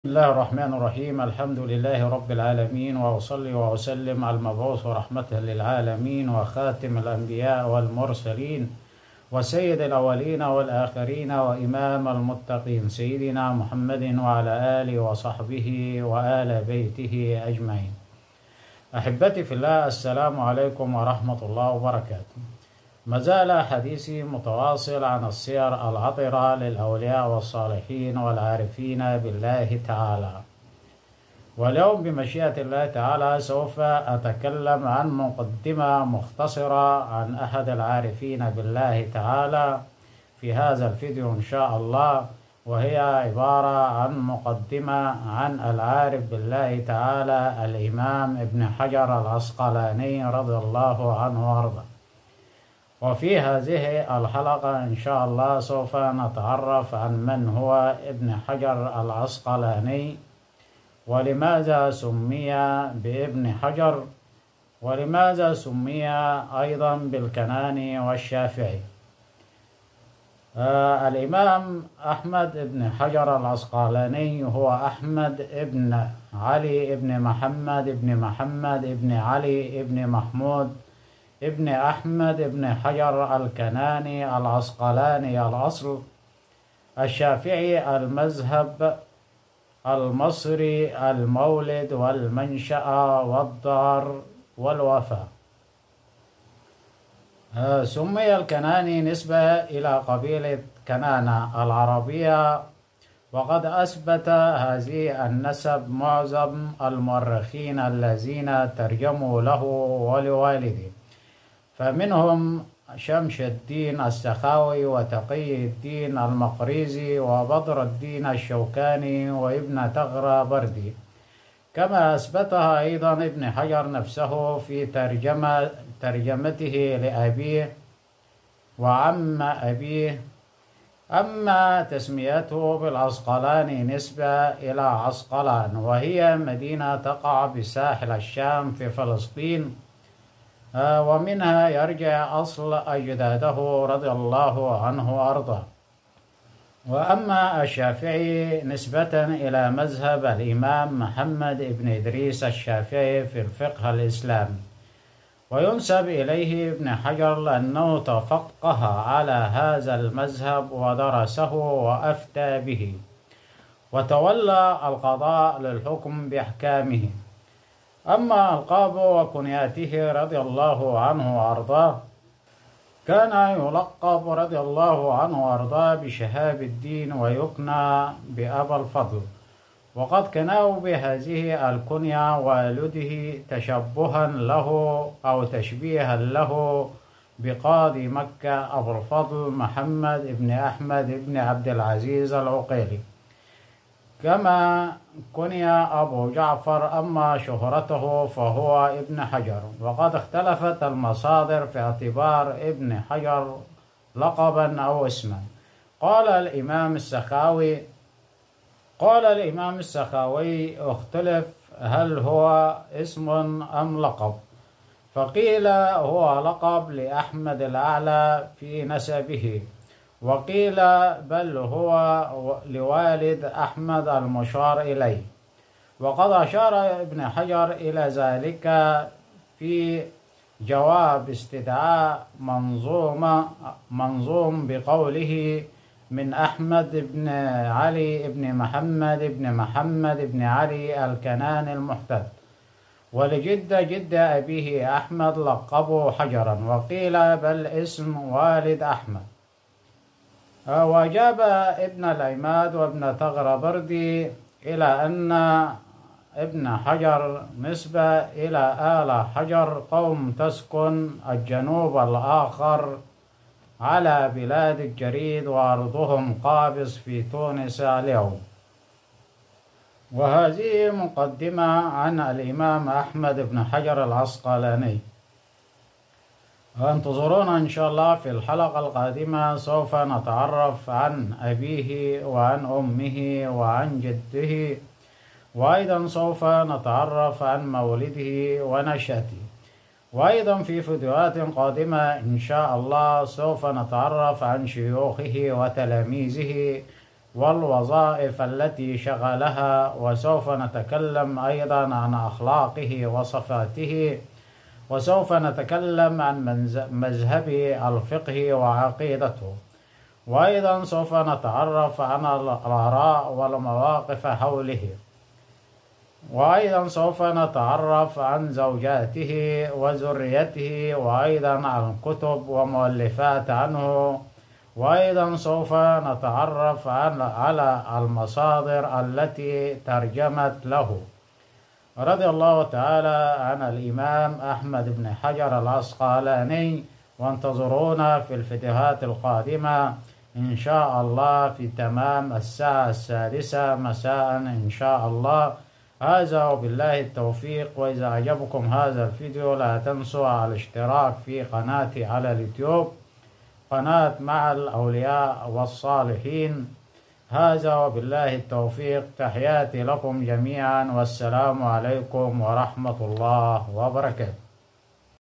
مقدمة عن الإمام احمد ابن حجر العسقلاني الكناني الشافعي
مقدمة عن الإمام أحمد ابن حجر العسقلاني  وهو  أحمد بن علي بن محمد بن محمد بن علي بن  محمود  بن أحمد  بن حجر الكناني  العسقلاني  الأصل  الشافعي  المذهب  المصري  المنشأ  والدار  والوفاه
والكناني  نسبه الي قبيله  كنانه  العربية  وقد اثبت  هذا  النسب  معظم  المؤرخين  الذين  ترجموا له ولوالده  فمنهم  شمس الدين  السخاوي  وتقي الدين  المقريزي  وبدر الدين  الشوكاني  وابن تغري بردي  كما اثبتها ابن حجر  نفسه  في ترجمته  لابيه  وعم ابيه
والعسقلاني  نسبه الي عسقلان وهي مدينة  تقع  بساحل  الشام  في  فلسطين  ومنها اصل اجداده
والشاقعي  نسبه  الي مذهب  الإمام  محمد بن  إدريس  الشافعي  في الفقه  الإسلامي  وينسب  اليه  ابن حجر  لانه تفقه  علي هذا  المذهب  ودرسه  وافتي  به وتولي القضاء  للحكم  باحكامه
لقبه وكنيته، ،،،،،،كان يلقب  بشهاب الدين  ويكني  ابا الفضل  وقد كنناه  بهذه  الكنيه  والده  تشبها  بقاضي  مكه  ابوالفضل  محمد  بن أحمد بن  عبدالعزيز  العقيلي  كما كني ايوجعفر  اما شهرته فهو ابن حجر
وقد اختلفت  المصادر  في اعتبار  ابن حجر  لقبا  او اسم  قال الإمام  السخاوي
اختلف  هل اسم  او لقب فقيل هو لقب  لأحمد الأعلي  في نسبه  وقيل بل  هو اسم لوالد  أحمد  المشار إليه
وقد أشار  ابن حجر  الي ذلك  في جواب  استدعاء  منظوم  بقوله من
أحمد  بن علي بن محمد بن محمد بن علي الكناني  المحتد  ولجد جد  أبيه  أحمد  لقبوا حجرا  وقيل اسم والد  أحمد
وذهب  ابن  العماد  وابن تغري بردي  الي ان حجر نسبه  الي ال حجر  قوم تسكن الجنوب  الآخر  علي بلاد  الجريد وارضهم  قابس  في  تونس  اليوم
هذا  وبالله التوفيق